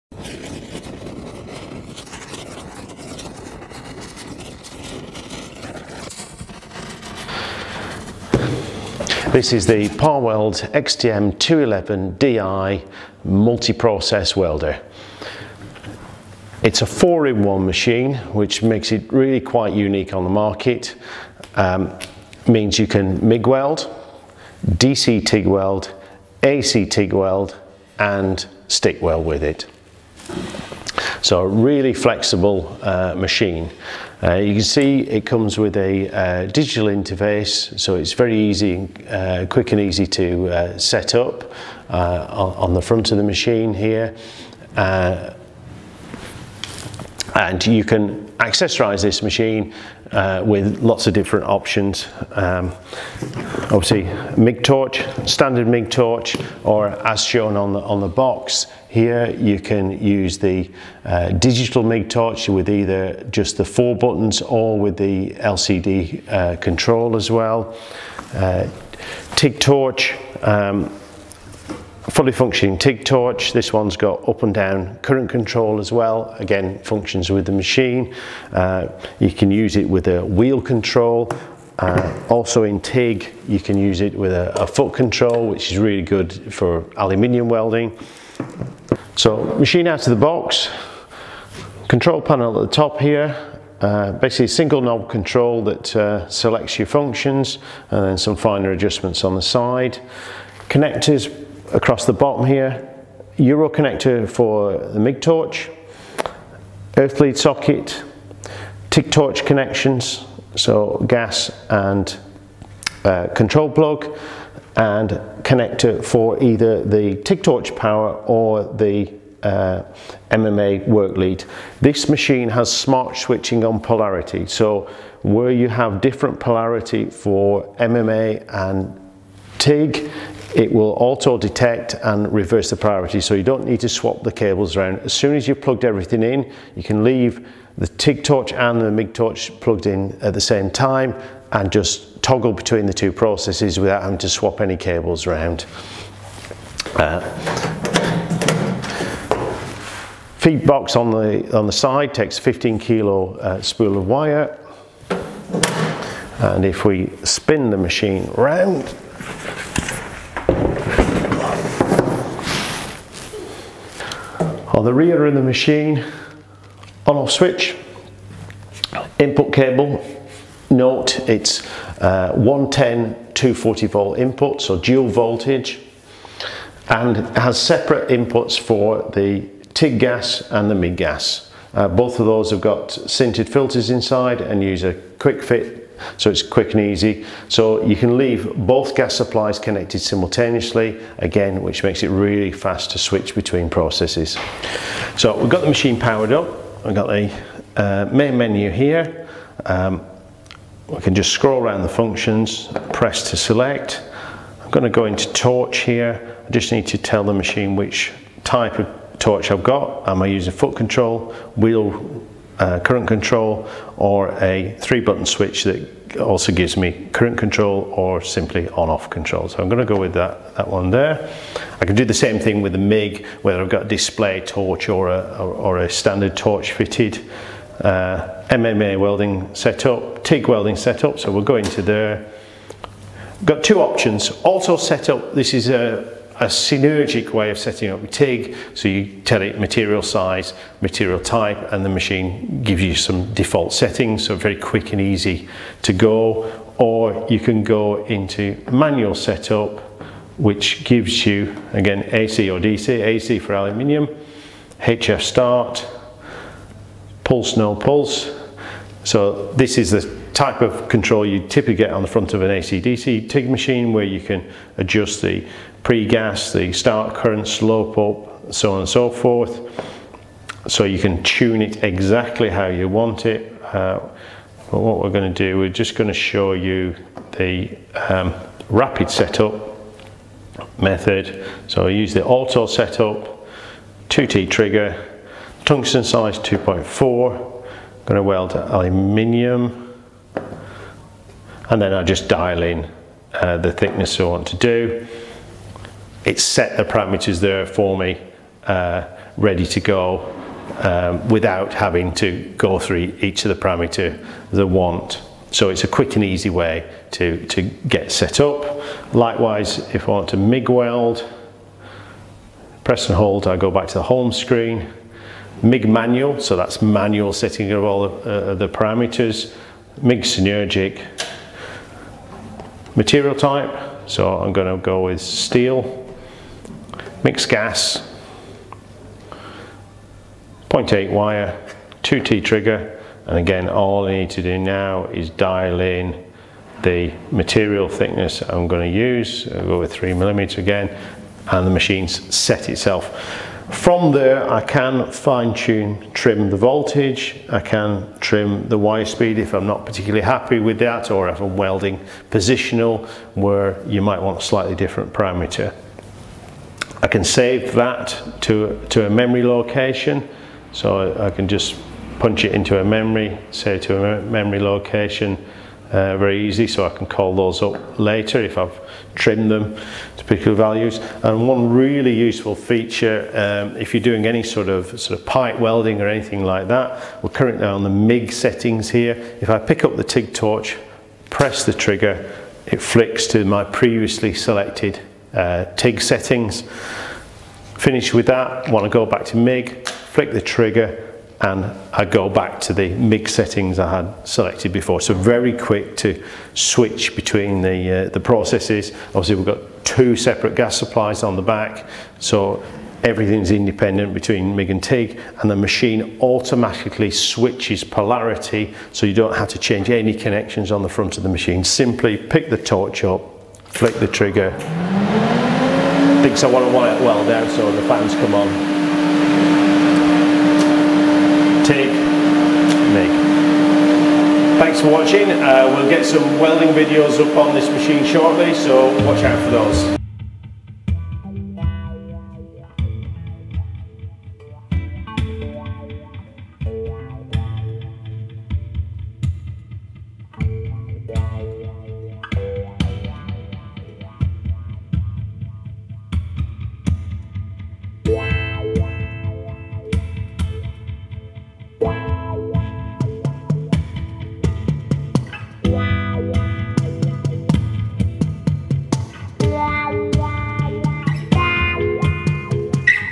XTM 211Di 4 in 1 Multi-Process Inverter Welder from Parweld
The XTM 211Di MIG Inverter from Parweld has 4 different processes available to the user making it ideal for the welding of Steels, Stainless Steels and Aluminium using the MIG, AC TIG, DC TIG or MMA process. The machine uses auto polarity switching so there is no need to change the work lead connections on the front of the machine. Instant swapping between the MIG and TIG processes with one touch of the torch trigger makes it well suited to pipe applications or multi-process workshops. The multi-language digital user interface rapidly gets you to the correct machine setting with minimal process knowledge, on-site or in the workshop.